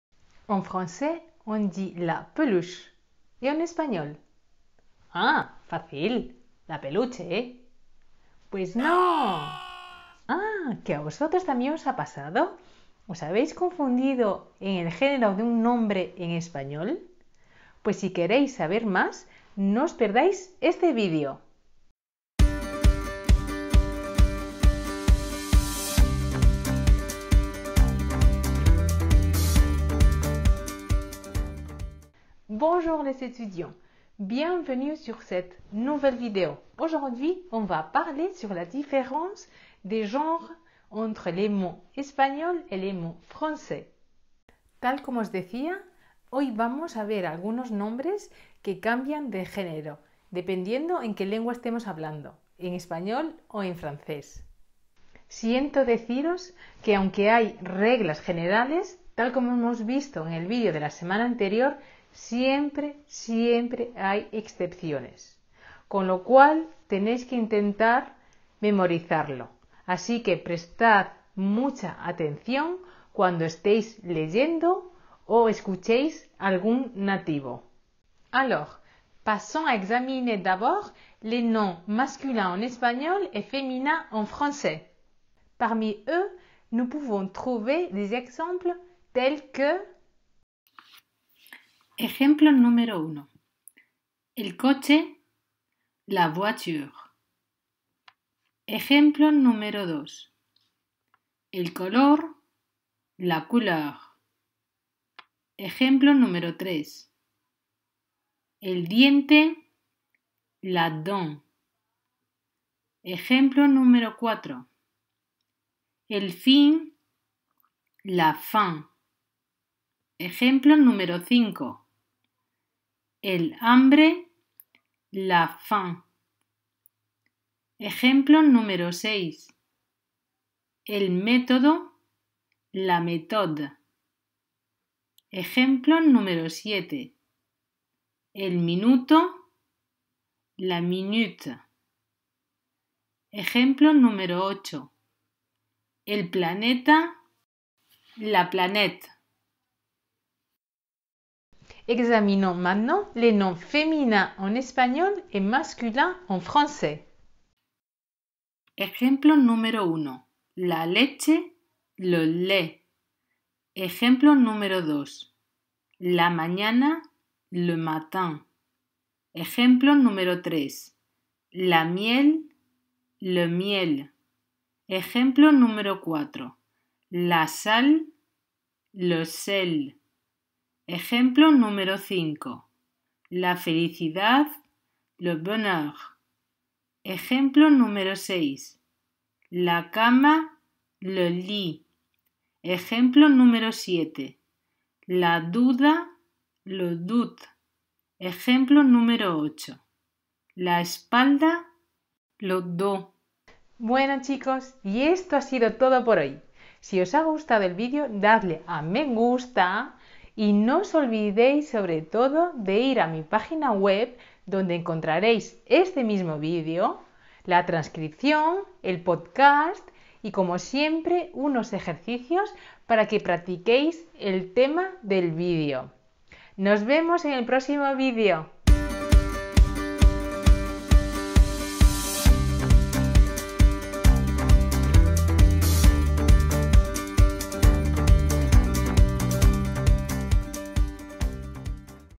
La différence de genre des noms en espagnol et en français
01:18 Noms masculin en espagnol et féminin en français
02:32 Noms féminin en espagnol et masculin en français

Aujourd'hui, nous allons voir des noms qui changent de genre selon la langue que nous parlons, en français ou en espagnol.
Je suis désolé de dire que bien qu'il y ait des règles générales comme nous l'avons vu dans la vidéo de la semaine dernière, il y a toujours des exceptions et nous devrons donc essayer d'apprendre le genre en faisant très attention quand nous lisons ou entendons quelqu'un parler.
Passons d'abord aux noms masculins en espagnol et féminins en français.

Parmi eux, nous pouvons trouver des exemples tels que :

9. El coche / la voiture
10. El color / la couleur
11. El diente / la dent
12. El fin / la fin
13. El hambre / la faim
14. El método / la méthode
15. El minuto / la minute
16. El planeta / la planète

Examinons maintenant les noms féminins en espagnol et les noms masculins en français.

9. La leche / le lait
10. La mañana / le matin
11. La miel / le miel
12. La sal / le sel
13. La felicidad / le bonheur
14. La cama / le lit
15. La duda / le doute
16. La espalda / le dos